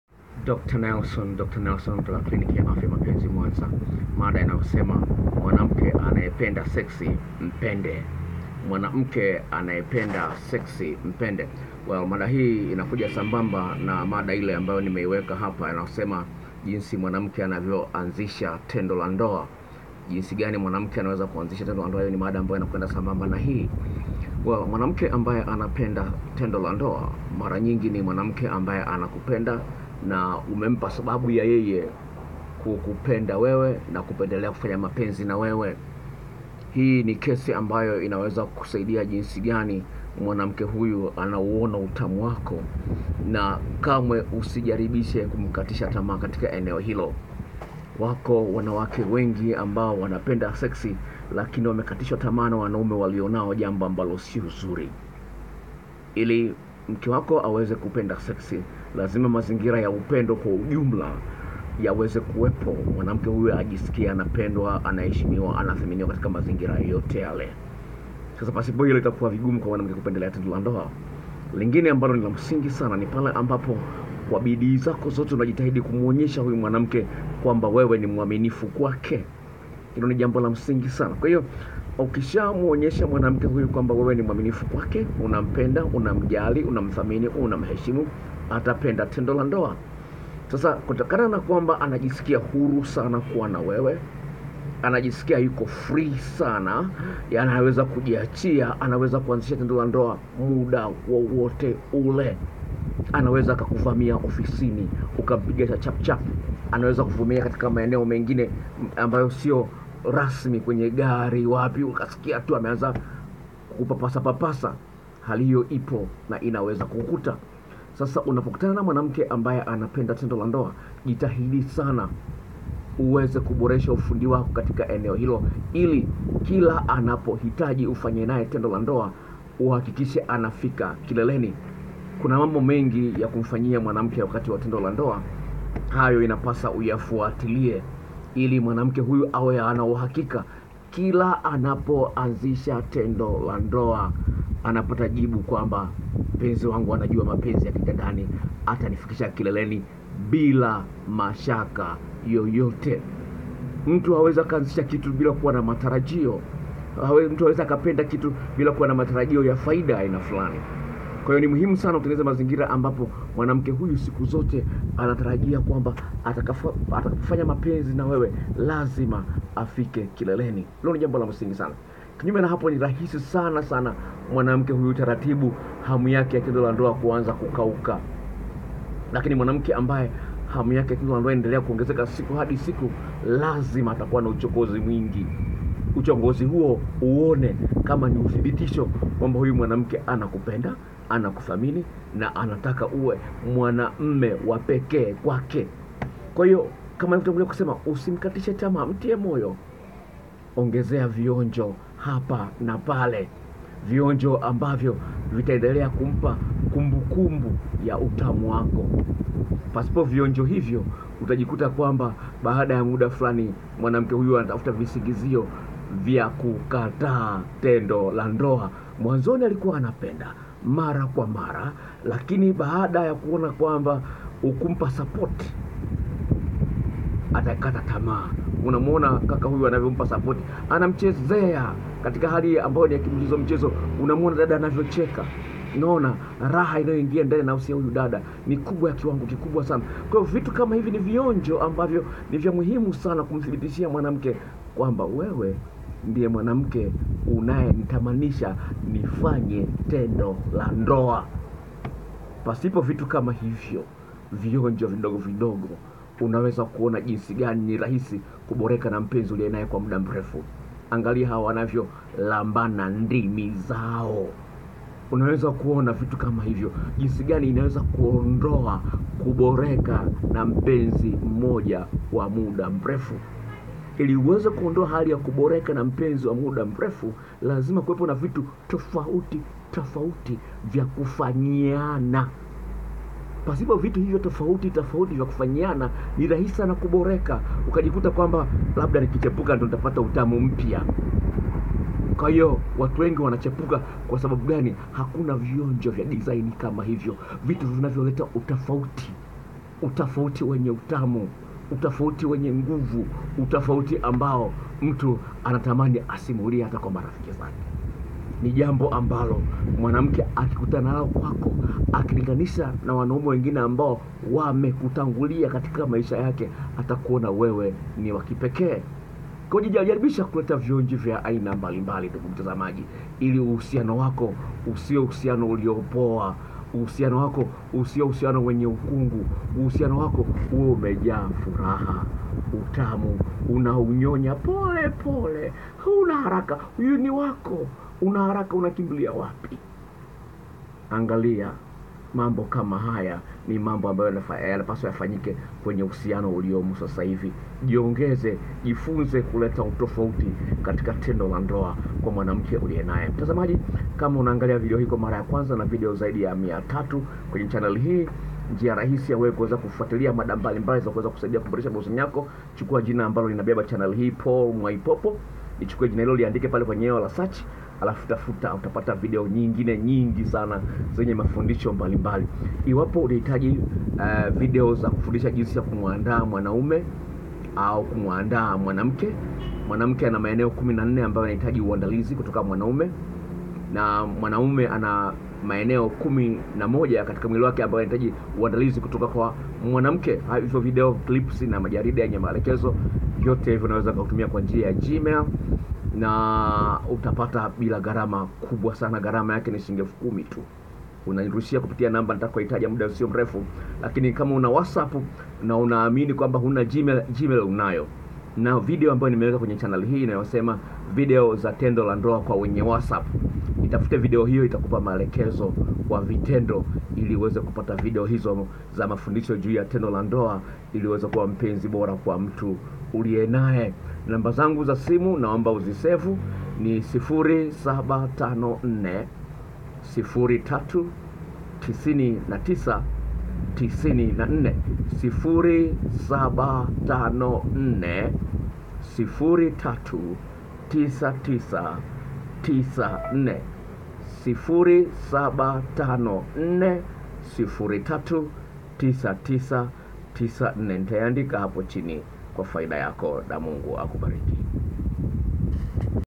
MKE akipenda tendo la ndoa mpende zaidi BY DR PAUL NELSON
Usiwe mwepesi wa kulaumu mkeo kwa mapungufu yake iwapo anajitahidi kukupa hitaji la kipekee la tendo la ndoa,shirikianeni kuondoa makunyanzi yoyote katika uhusiano wenu